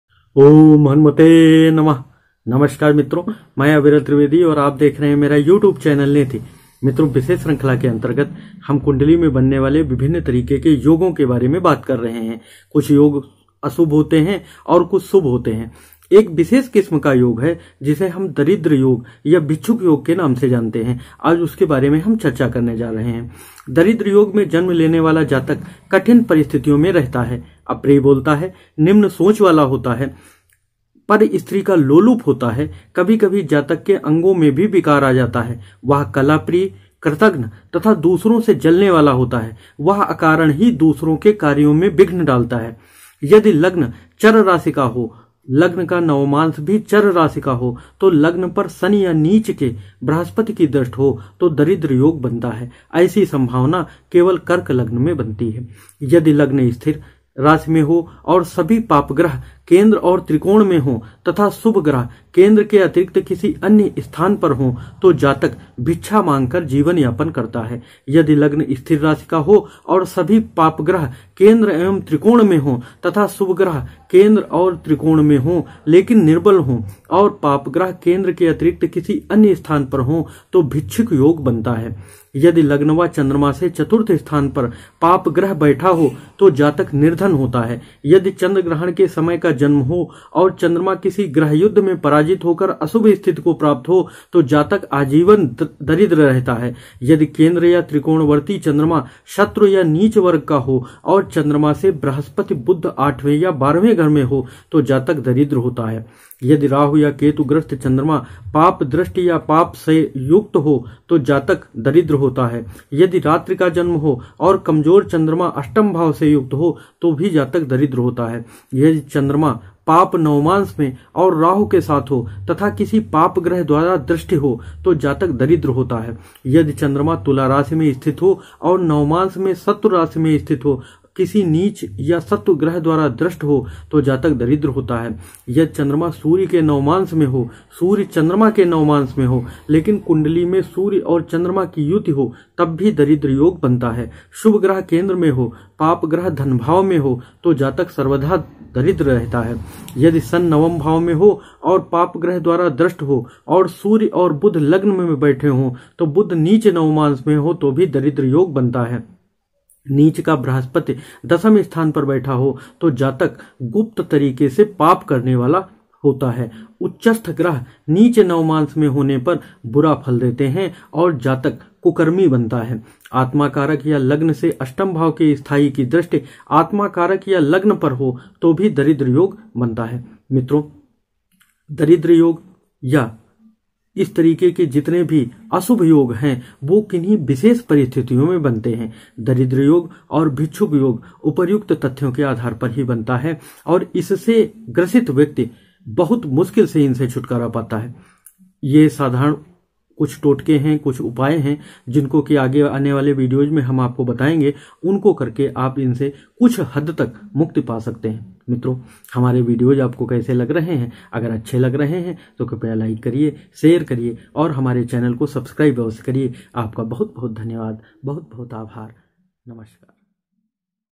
दरिद्र/भिक्षुक ,योग कुंडली का कष्टकारी योग
दरिद्र योग में जन्म लेने वाला जातक कठिन परिस्थितियों में रहता है ,अप्रिय बोलता है, निम्न सोच वाला होता है ,पर स्त्री लोलुप होता है ,कभी-कभी जातक के अंगों में भी विकार आ जाता है ,वह कलह प्रिय होता है, क्रतघ्न तथा दूसरों से जलने वाला होता है, वह अकारण ही दूसरों के कार्यों में विघ्न डालता है।
        यदि लग्न चर राशि का हो लग्न का नवमांश भी चर राशि का हो तो ,लग्न पर शनि या नीच के बृहस्पति की दृष्टि हो तो दरिद्र योग बनता है ,ऐसी संभावना केवल कर्क लग्न में बनती है।
       यदि लग्न स्थिर राशि में हो और सभी पाप ग्रह केंद्र और त्रिकोण में हो ,तथा शुभ ग्रह केंद्र के अतिरिक्त किसी अन्य स्थान पर हो तो जातक भिक्षा मांगकर जीवन यापन करता है ।
       यदि लग्न स्थिर राशि का हो, और सभी पाप ग्रह केंद्र एवं त्रिकोण में हो तथा शुभ ग्रह केंद्र और त्रिकोण में हों लेकिन निर्बल हो ,और पाप ग्रह केंद्र के अतिरिक्त किसी अन्य स्थान पर हो तो ,भिक्षुक योग बनता है।
         यदि लग्न व चंद्रमा से चतुर्थ स्थान पर पाप ग्रह बैठा हो तो जातक निर्धन होता है।
        यदि चंद्र ग्रहण के समय का जन्म हो और चंद्रमा किसी ग्रह युद्ध में पराजित होकर अशुभ तो जातक  आजीवन दरिद्र रहता है ।
       यदि केंद्र या त्रिकोण वर्ती चंद्रमा नीच वर्ग और चंद्रमा से वृहस्पति,  बुध आठवें या बारहवें घर में हो तो जातक दरिद्र होता है ।
        यदि राहु या केतु ग्रह चंद्रमा पाप द्रष्टि या पाप संयुक्त हो तो जातक दरिद्र होता है ।
      यदि रात्रि का जन्म हो और कमजोर चंद्रमा अष्टम भाव से युक्त हो तो जातक दरिद्र होता है ।
      यदि चंद्रमा पाप नवमांश में हो और राहु के साथ हो तथा किसी पाप ग्रह द्वारा दृष्ट हो तो जातक दरिद्र होता है।
        यह चंद्रमा तुला राशि में स्थित हो और नवमांश में  शत्रु राशि में स्थित हो और किसी नीच या शत्रु ग्रह द्वारा दृष्ट हो तो जातक दरिद्र होता है ।
         यदि चंद्रमा सूर्य के नवमांश में हो, सूर्य चंद्रमा के नवमांस में हो, लेकिन कुंडली में सूर्य और चंद्रमा की युति हो तो दरिद्र योग बनता है।
       शुभ ग्रह केन्द्र हो  पापग्रह धनभाव में हो तो जातक सर्वथा दरिद्र रहता है।
         यदि शनि नवम भाव में हो और पाप ग्रह द्वारा दृष्ट हो और सूर्य और बुध लग्न में बैठे हो तो ,बुध नीच नवमांश में हो तो दरिद्र योग बनता है ।
        नीच का बृहस्पति दशम  स्थान पर बैठा हो तो जातक गुप्त तरीके से पाप करने वाला होता है ।
      उच्चस्थ ग्रह नीच नवमांश में होने पर बुरा फल देते हैं और जातक को कुकर्मी बनाते है ।
       आत्मा कारक या लग्न से अष्टम भाव के स्वामी की द्रष्टि आत्मा कारक या  लग्न पर हो तो दरिद्र योग बनता है।